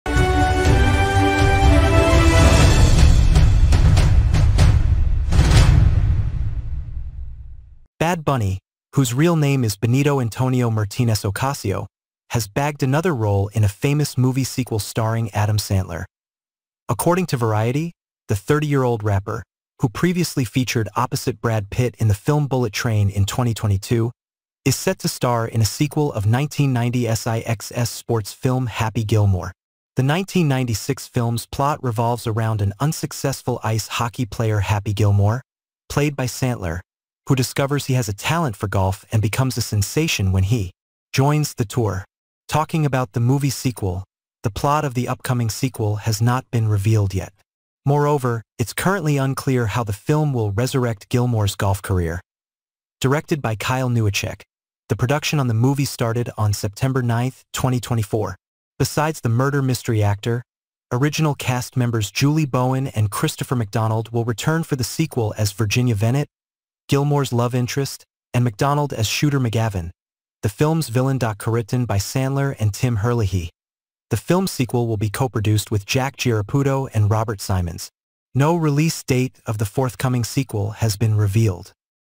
Bad Bunny bags role in ‘Happy Gilmore' sequel| Us Entertainment News
Trending Topic

Topic Cover